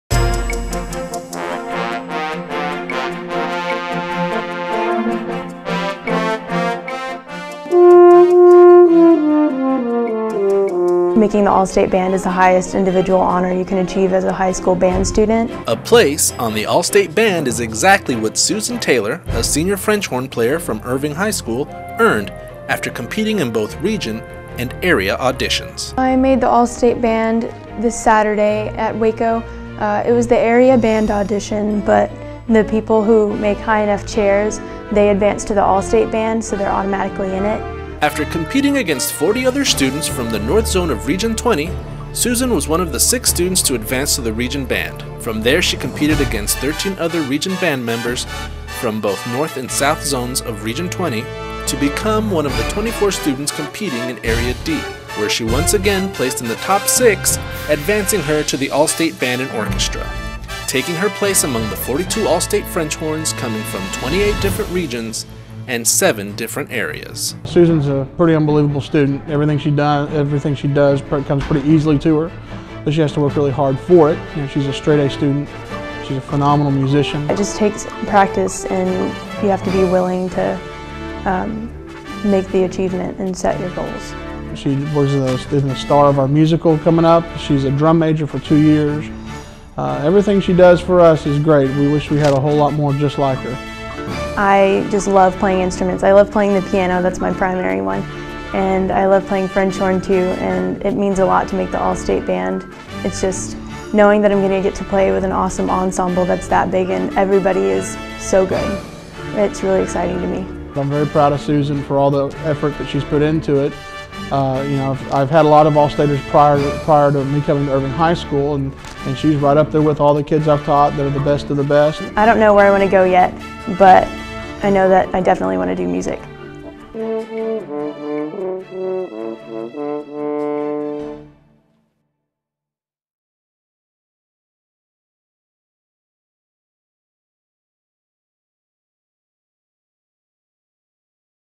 Irving High Senior Susan Taylor Makes All-State Band
Susan Taylor, senior French horn player at Irving High School, earned a seat in the Texas All-State band following auditions in Waco January 8.